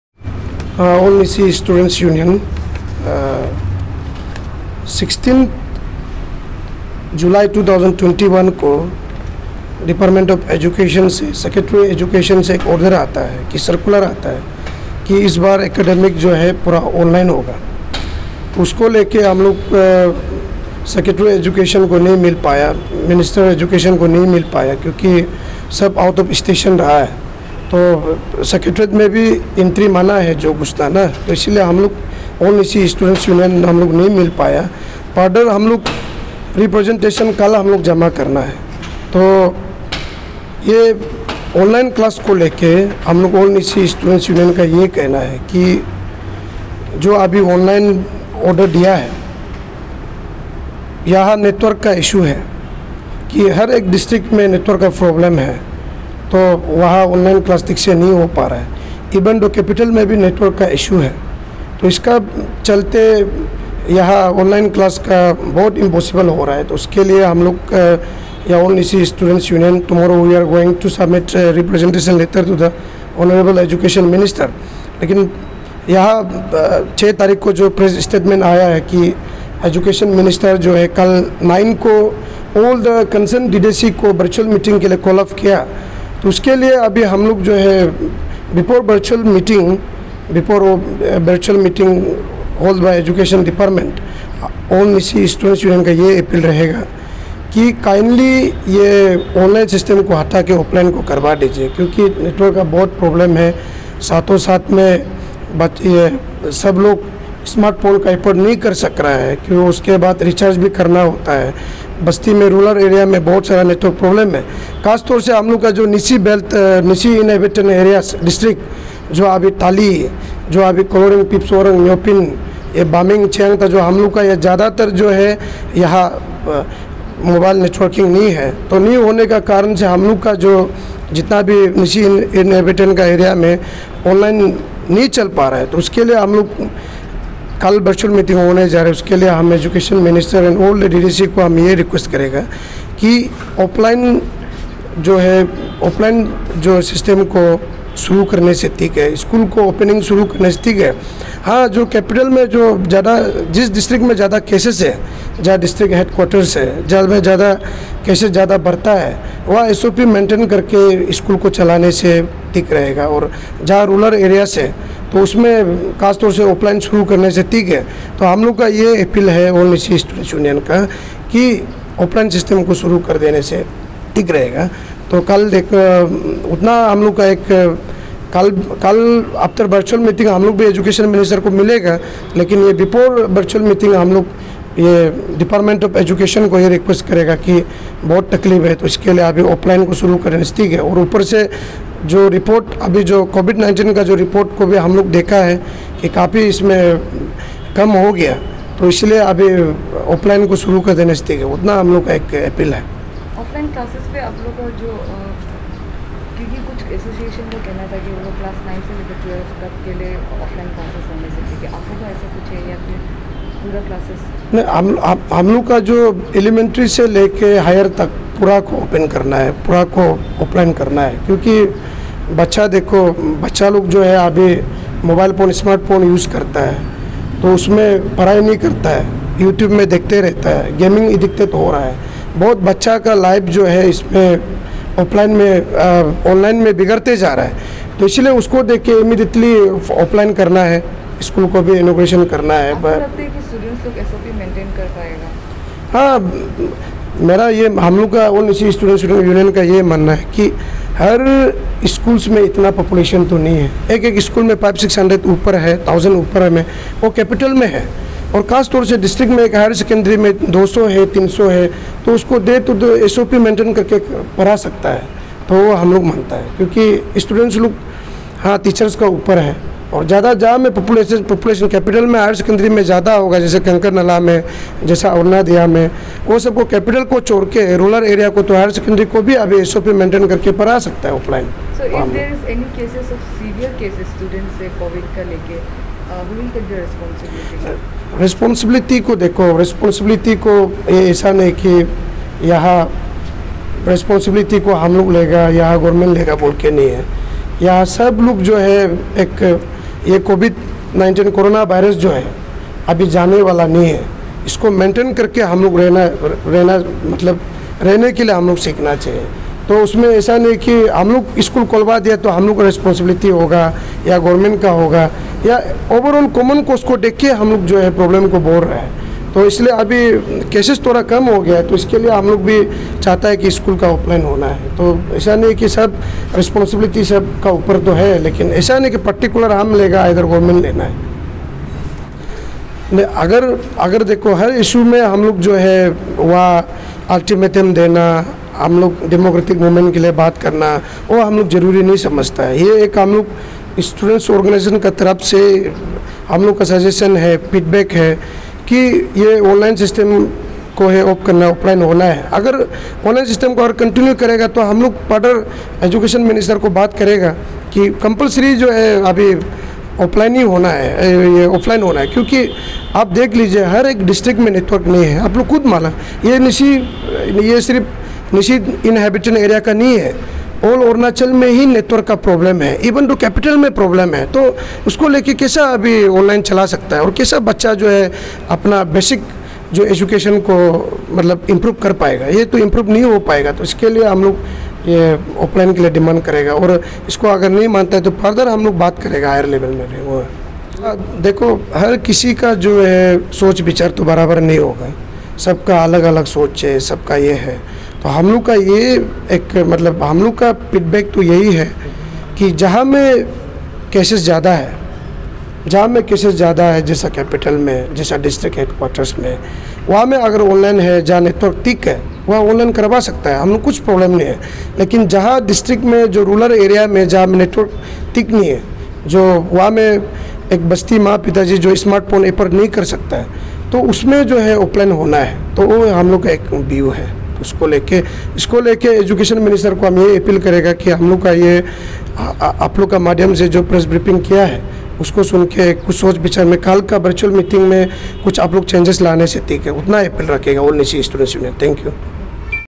ANSU appealed to the education minister and all the DDSE to conduct offline classes.
The All Nyishi Students' Union have appealed to the education minister and all the DDSE to conduct offline classes instead of online classes due to mobile network connectivity issues in the districts.